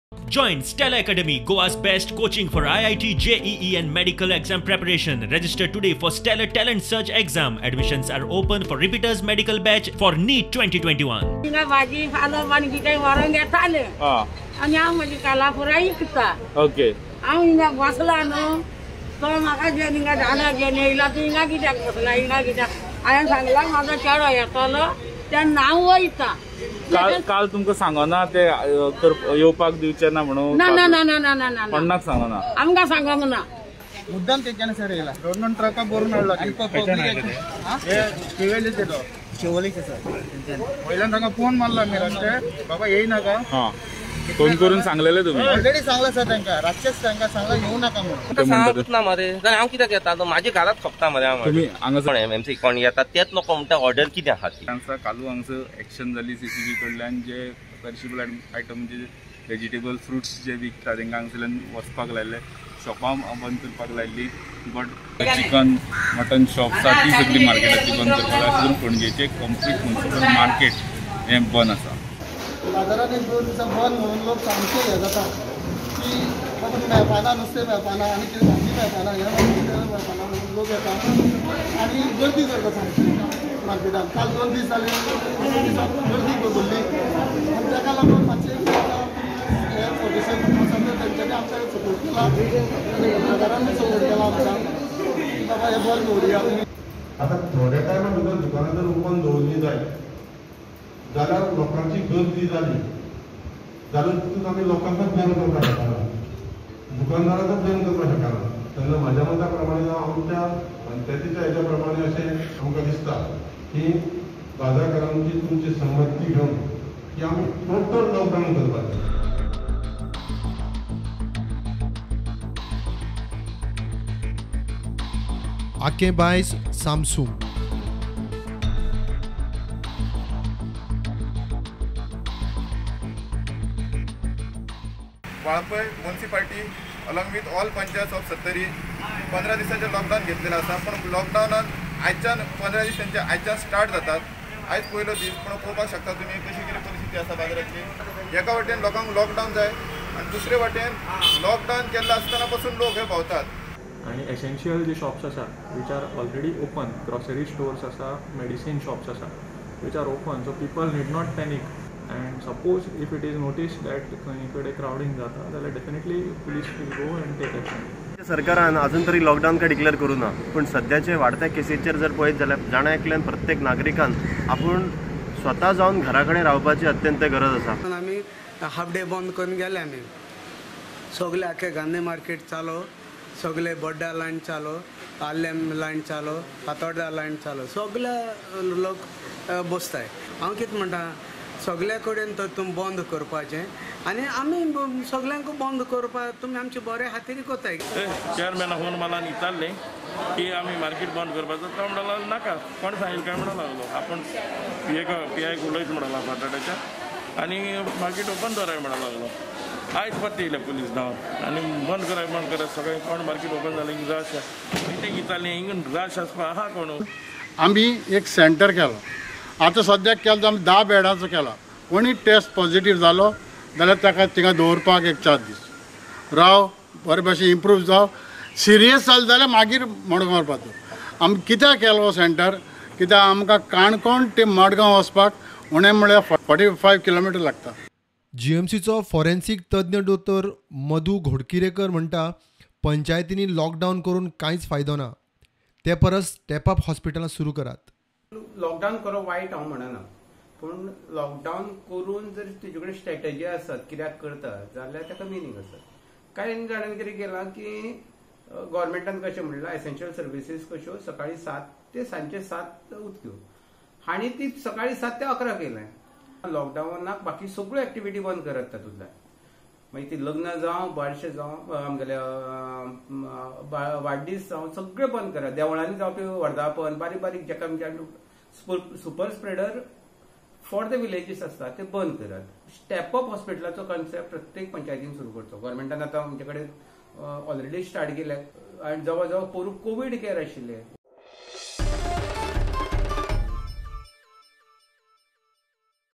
PANAJI MUNICIPAL MARKET SHUT FOR 2ND CONSECUTIVE DAY
As COVID-19 cases and mortalities are rising day by day, village panchayats across the state have opted for strict lockdowns. For the second consecutive day Panaji Municipal Market was shut by police. With restrictions in place it has been witnessed that at some places the buzz is come at a standstill however in other places the buzz of people is still been witnessed. Experts say that step-up hospitals should be set up.